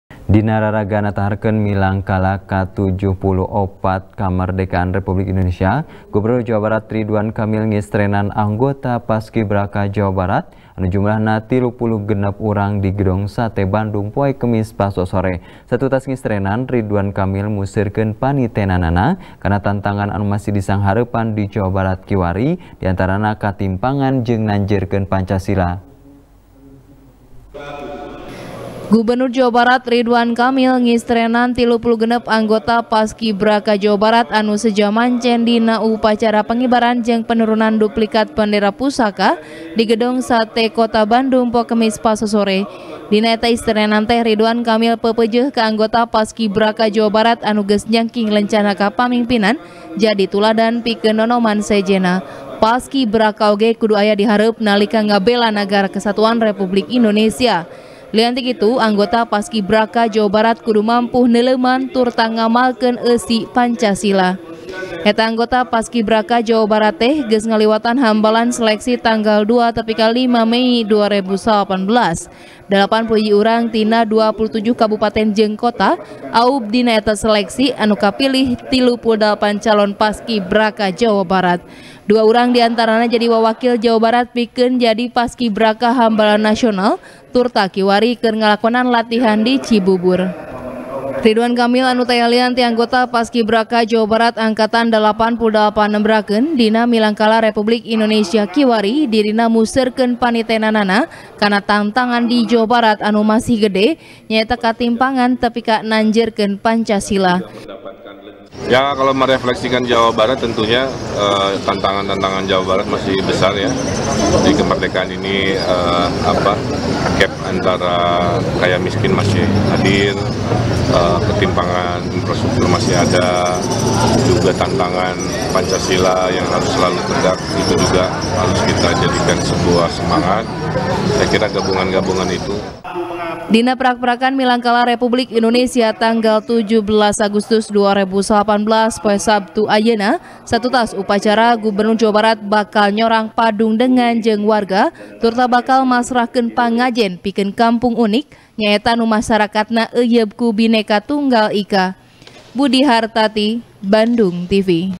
Ngistrenan Paskibraka, Ridwan Kamil museurkeun panitenan kana tantangan di Jabar | TP | BANDUNGTV
TP | BANDUNGTV | IWAN M RIDWAN | Gubernur Jawa Barat, Ridwan Kamil, ngistrenan 36 anggota Paskibraka Jabar, anu seja mancen dina upacara pengibaran jeung penurunan duplikat bendera pusaka, di gedong sate Bandung, poe kemis pasosore. Dina eta istrenan teh, ridwan kamil peupeujeuh ka anggota Paskibraka Jabar anu geus nyangking lencana kapamingpinan, jadi tuladan pikeun nonoman sejenna. Paskibraka oge kudu aya di hareup nalika ngabela negara kesatuan republik Indonesia. Lian ti kitu, anggota Paskibraka Jabar kudu mampu neuleuman turta ngamalkeun eusi pancasila.